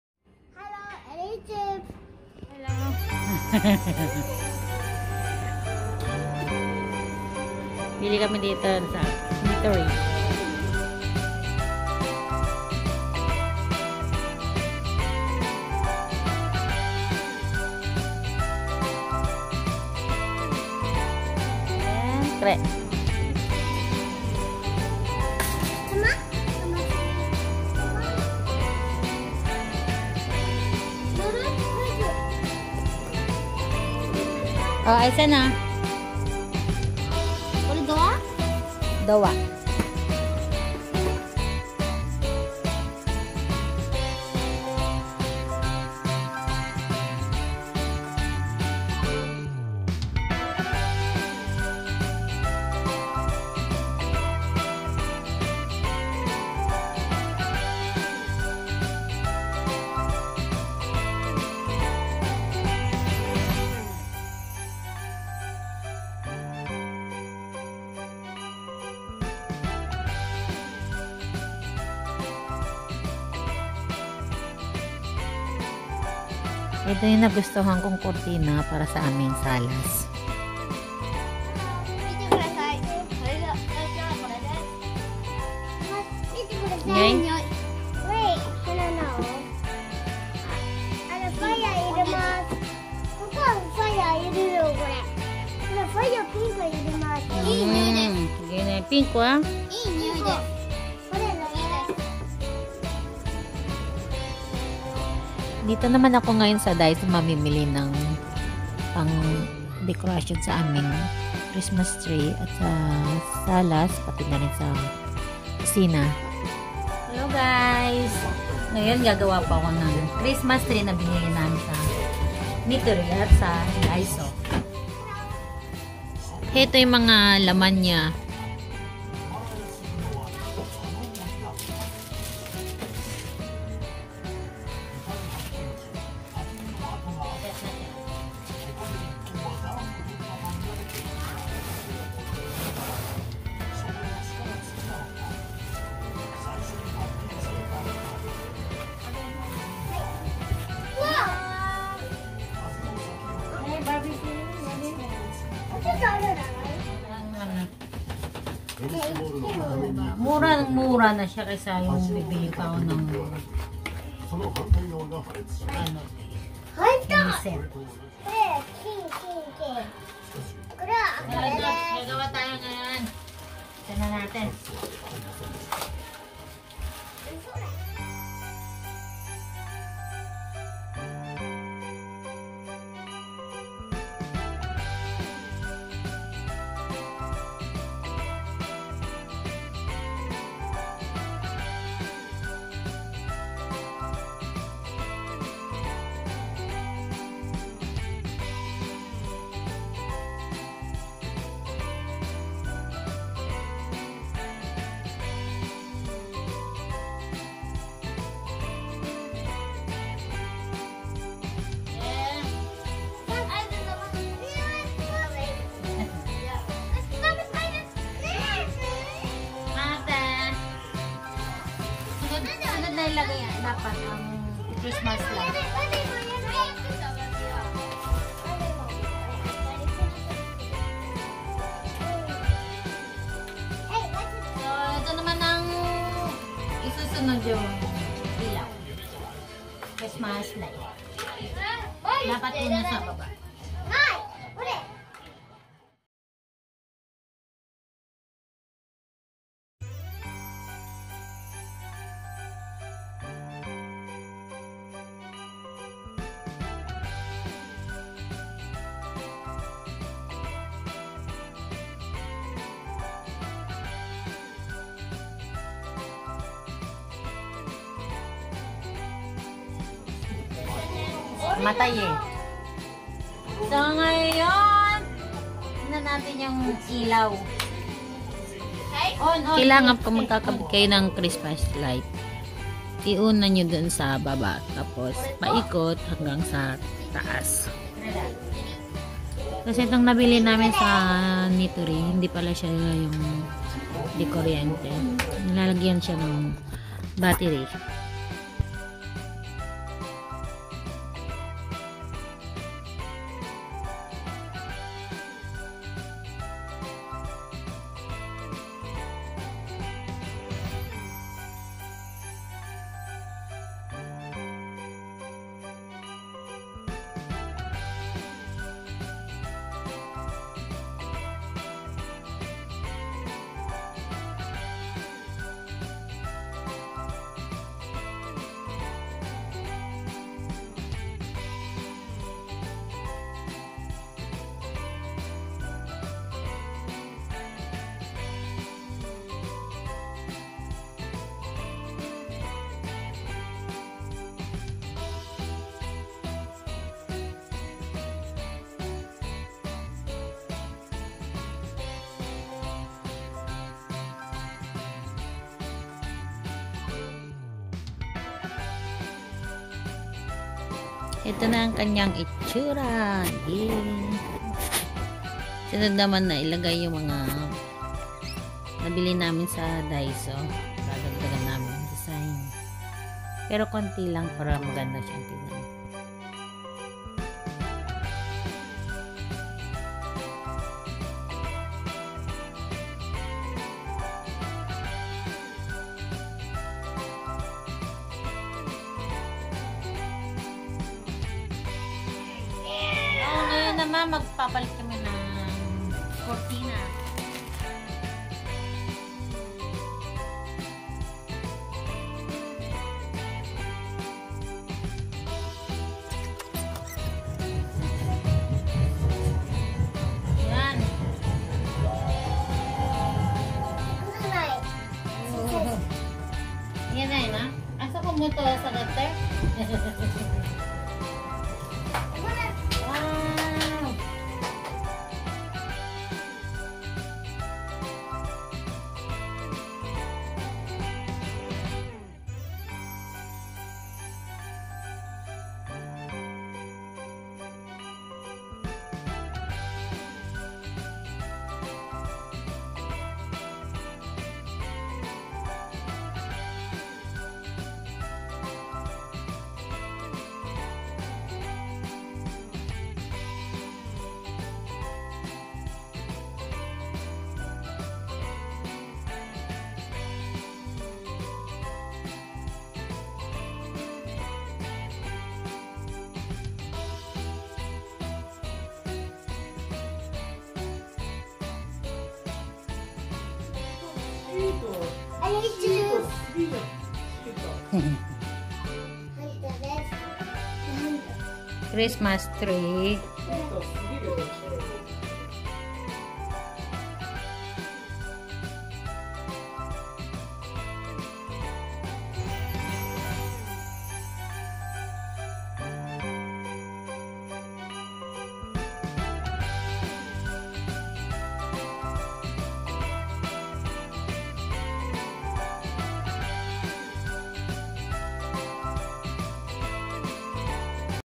christmastreedesign CHRISTMAS TREE DESIGN ON A BUDGET
our xmas tree design this year !!
gawing bongga ang simpleng set na nabili namin sa nitori
dagdagan lang ng daiso xmas ornament para magmukhang busog 😄
medyu bungal bungal kasi itsura kapag konti lang nakalagay.